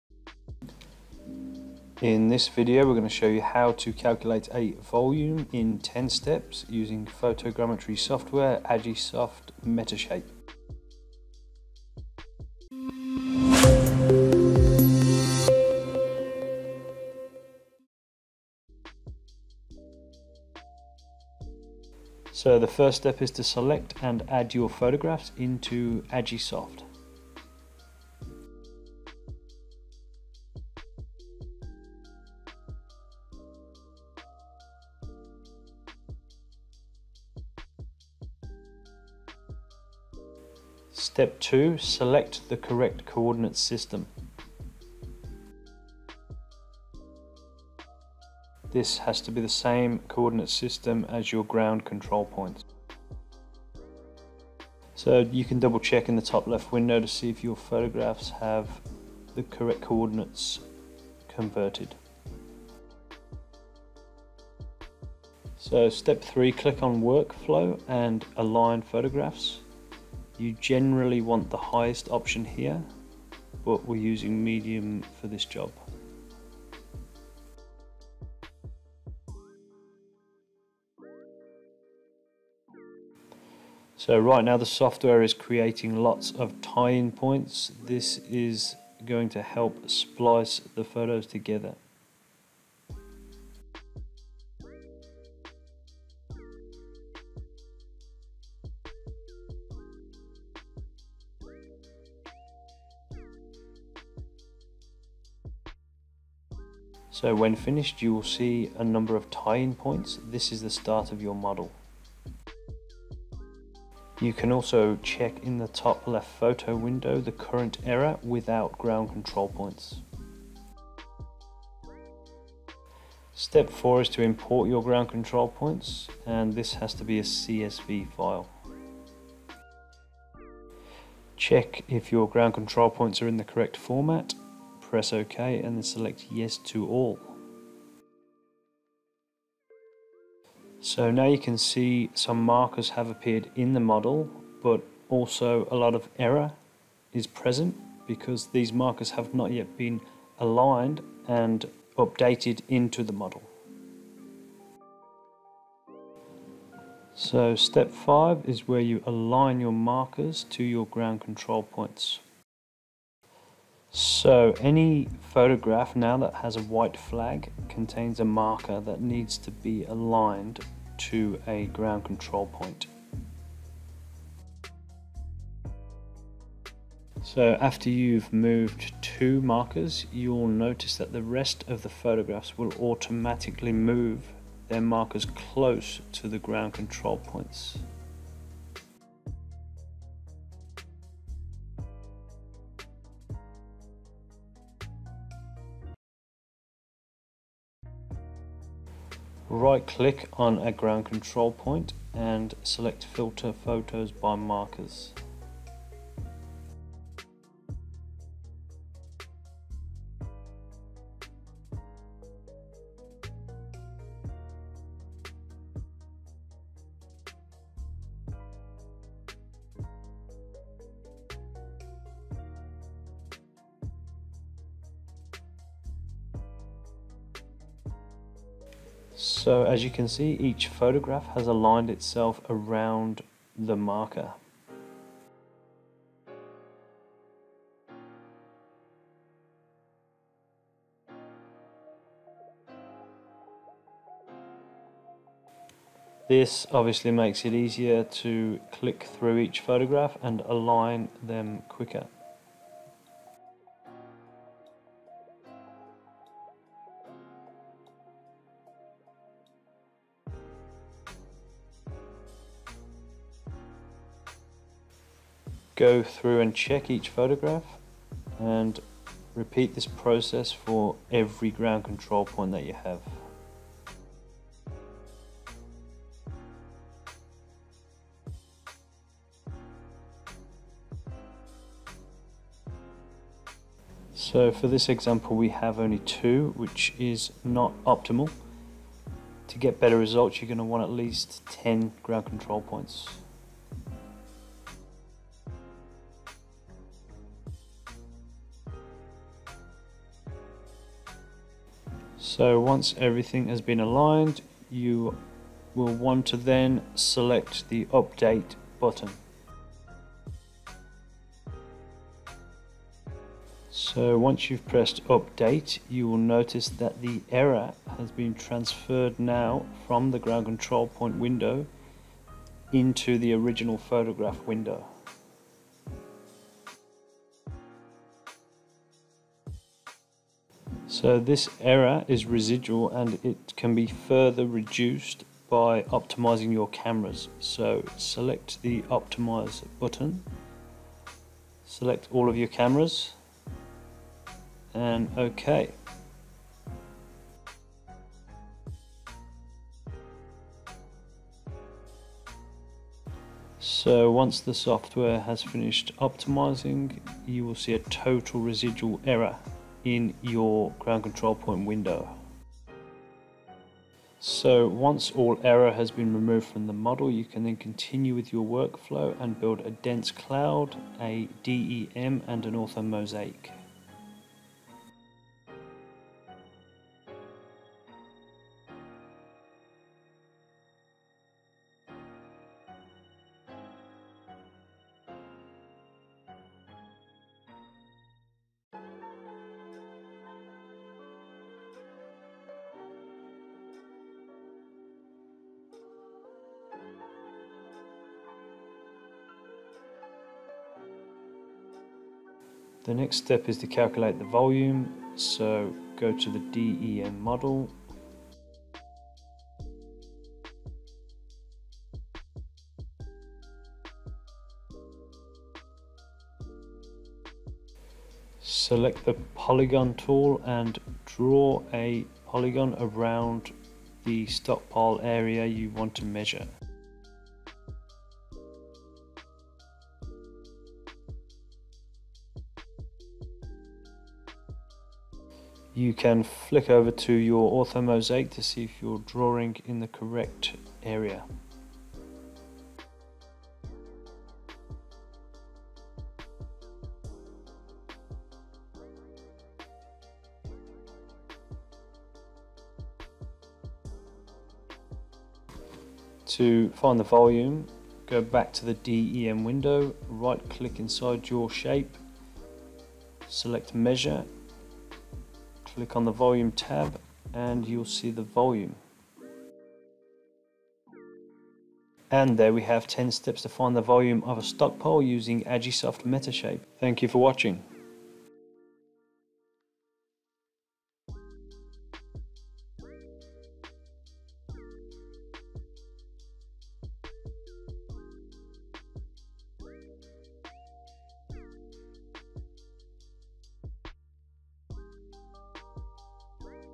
10 Steps to Calculate a Volume in Agisoft Metashape with GCPs | Aerial Surveying
Using Drone Photogrammetry Software Agisoft Metashape.

This Video Narrows down the 10 Easy Steps to Generate an Accurate Volume of a Stockpile, using Imported Ground Control Points.

The Video Outlines the Process from Alignment of Photos to Creation of a Dense Cloud, Digital Elevation Model and Orthomosaic.